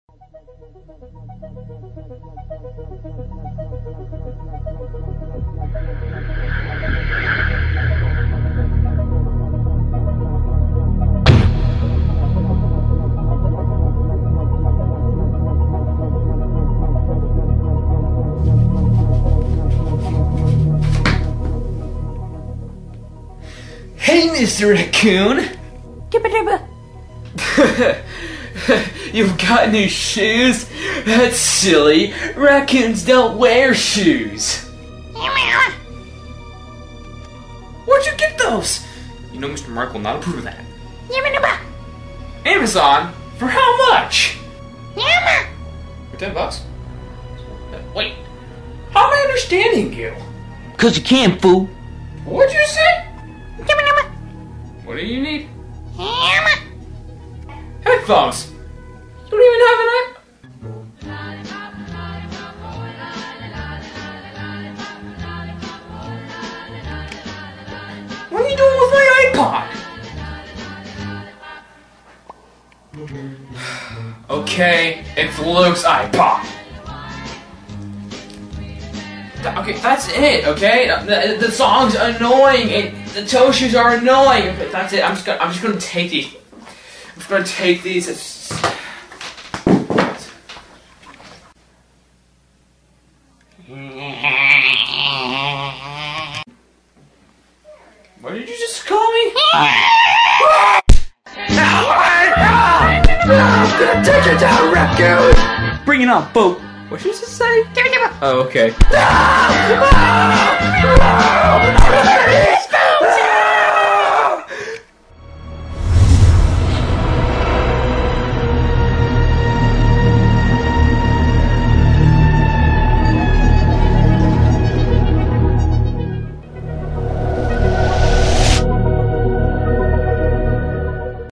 Mr. Racoon and the Ipod.
PLEASE COMMENT AND SUBSCRIBE!!!  (I KNOW ITS RANDOM AND STUDPID, ITS MADE THIS WAY TO BE FUNNY)   I made this with Adobe After Effects and Adobe Sound Booth for at our church: HighRidge Church.